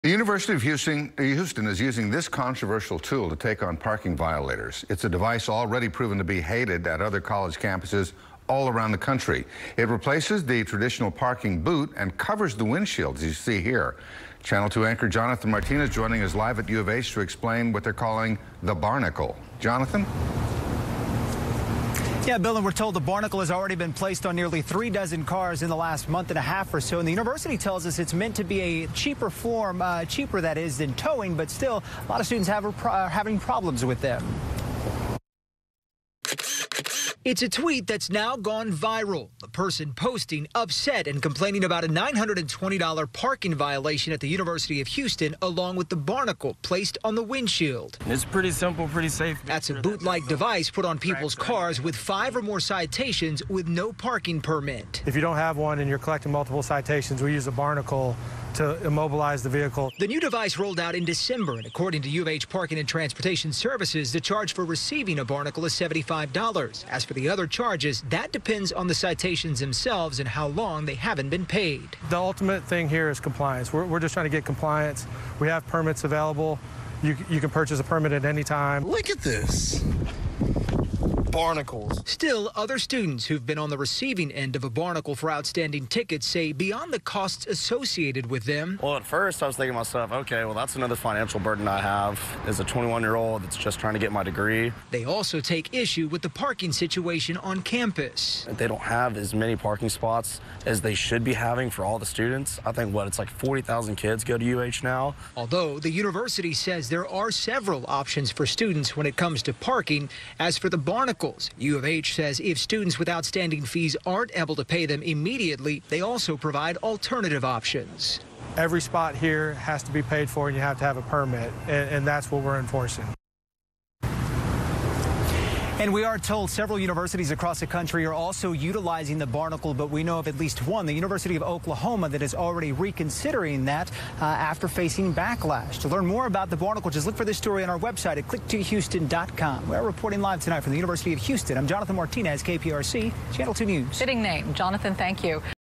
Meet ‘The Barnacle’: Twitter user complains about UH $920 windshield penalty, hilarious response...
The University of Houston is responding after a Twitter user complained about a $920 parking violation that included a windshield device akin to a tire boot dubbed “the barnacle.”